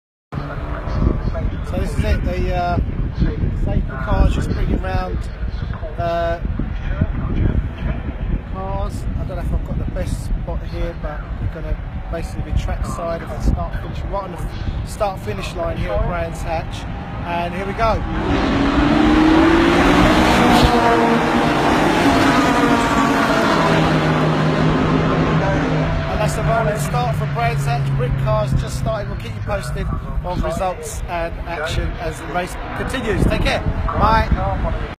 Brands Hatch 29/07/17 Britcar Endurance race start
Erkan takes us live to the start of Britcar Endurance Brands Hatch 29/07/17